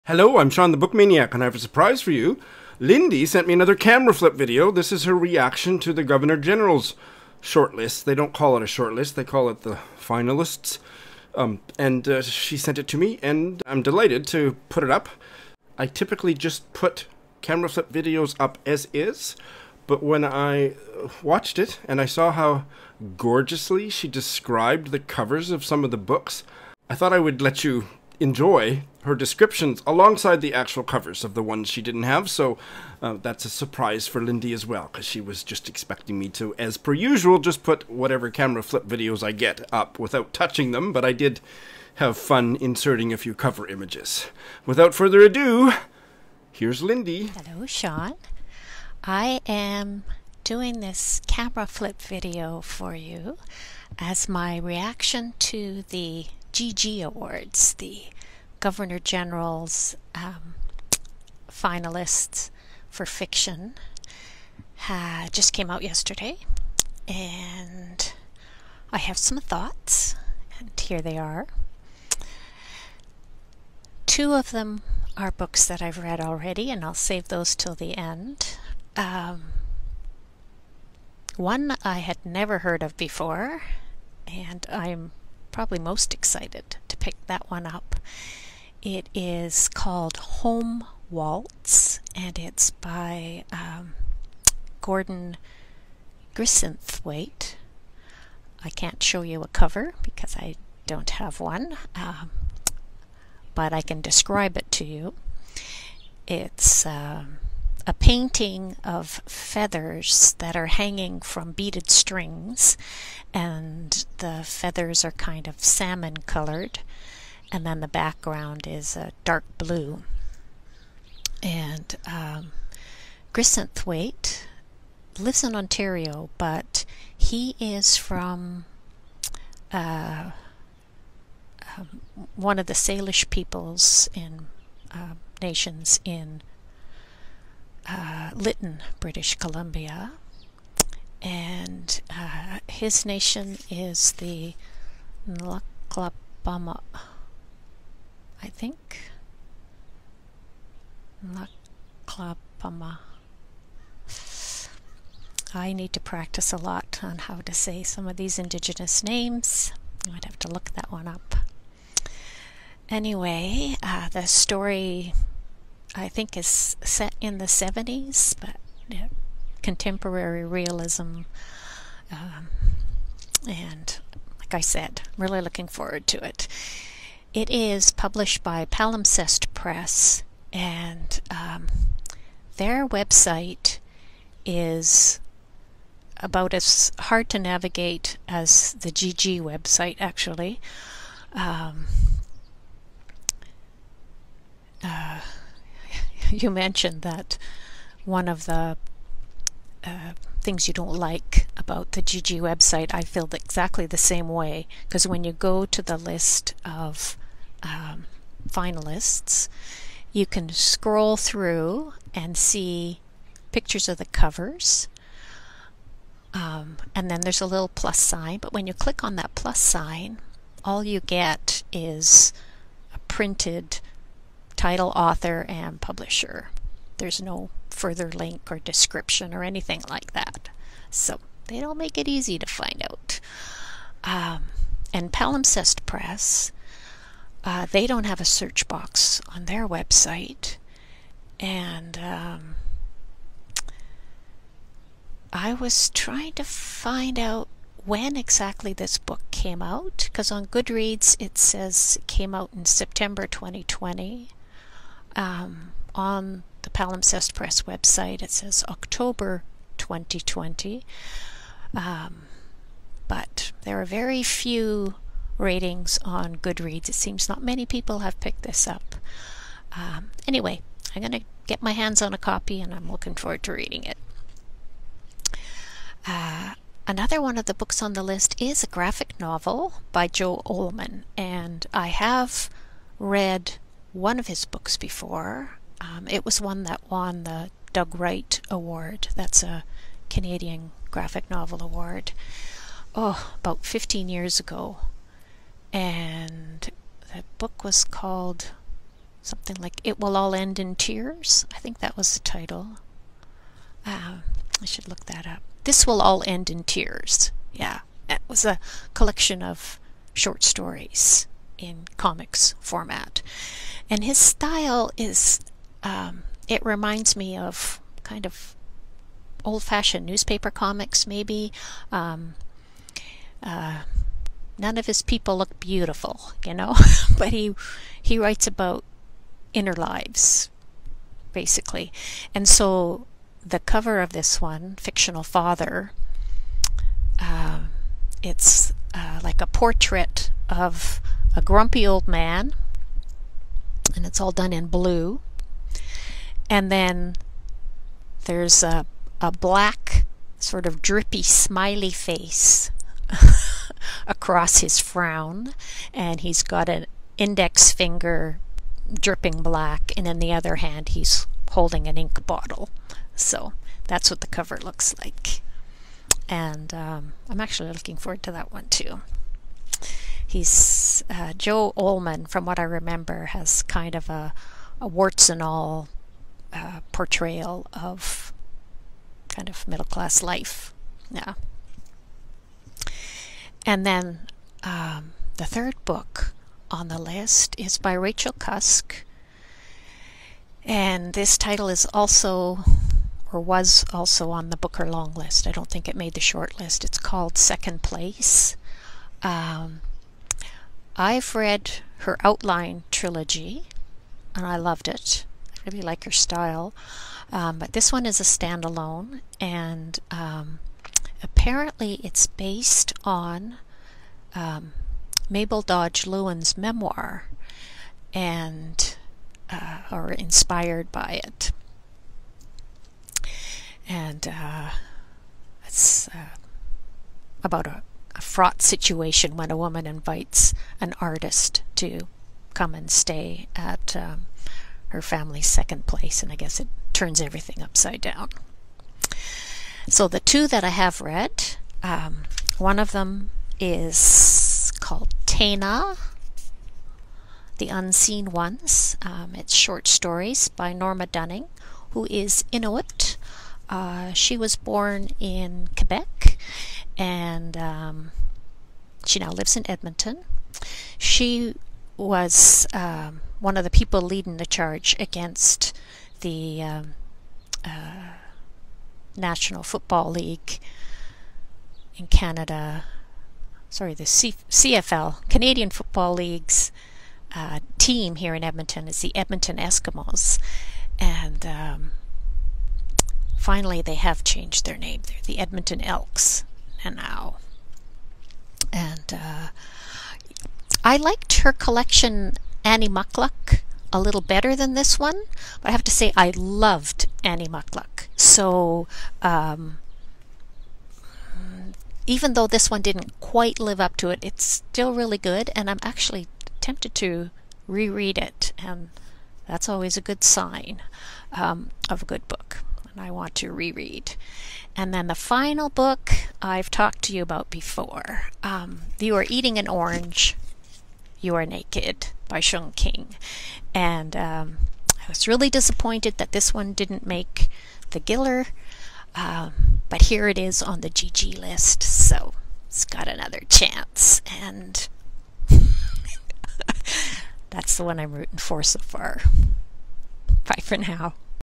Camera Flip: Lindy reacts to the Governor General's fiction prize shortlist
* Fictional Father by Joe Ollmann
* Home Waltz by G. A. Grisenthwaite
* Second Place by Rachel Cusk
* Tainna: The Unseen Ones by Norma Dunning
* You Are Eating an Orange. You Are Naked. by Sheung-King

I’m @shawnmooney on Litsy and on Twitter.
I’m shawnthebookmaniac on Instagram.